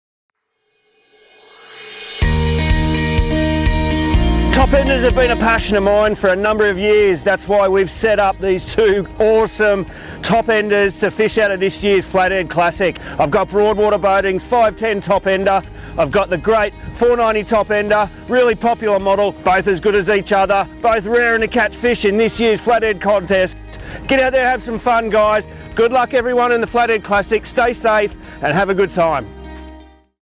2016 Flathead Classic Entries - Surf Coast Marine & Broadwater Boating Centre
Take a look at the 2016 Gold Coast Flathead Classic team entries for Broadwater Boating and Surf Coast Marine.